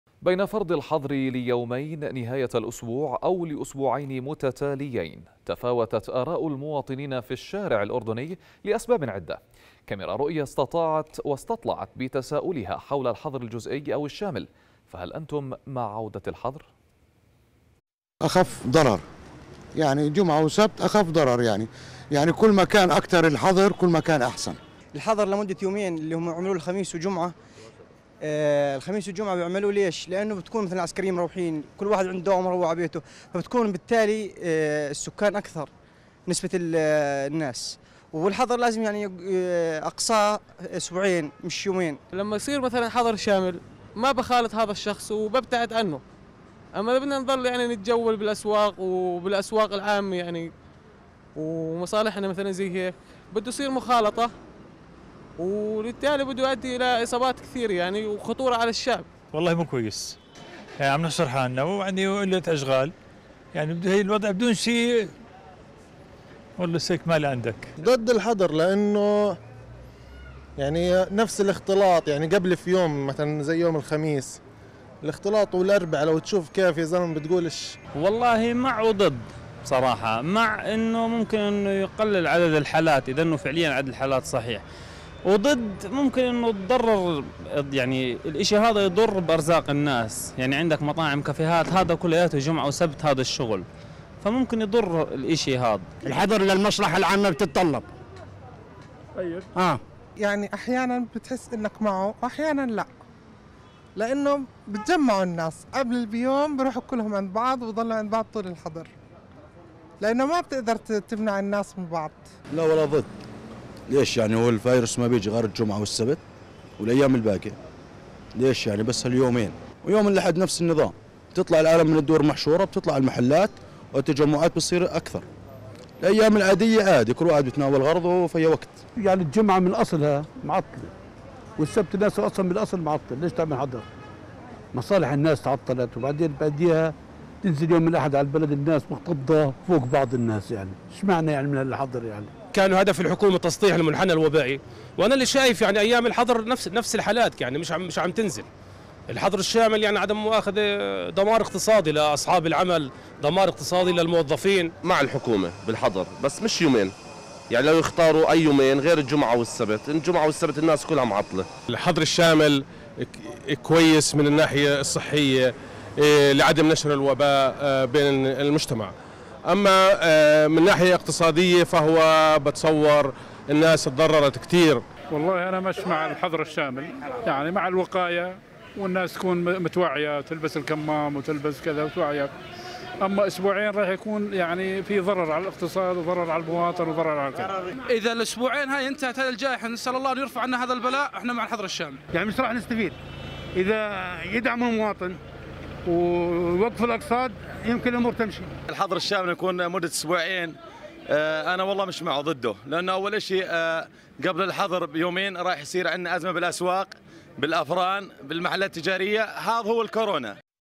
رؤيا تستطلع آراء المواطنين حول الحظر الجزئي أو الشامل "هل أنتم مع عودة الحظر"؟ - 12/10/2020
بين فرضِ الحظرِ ليومين نهايةَ الأسبوع أو لأسبوعين متتاليين، تفاوتت آراءُ المواطنينِ في الشارع الأردني لأسبابٍ عدة، كاميرا رؤيا استطلعت بتساؤلها حول الحظرِ الجزئي أو الشامل، فهل أنتم مع عودة الحظر.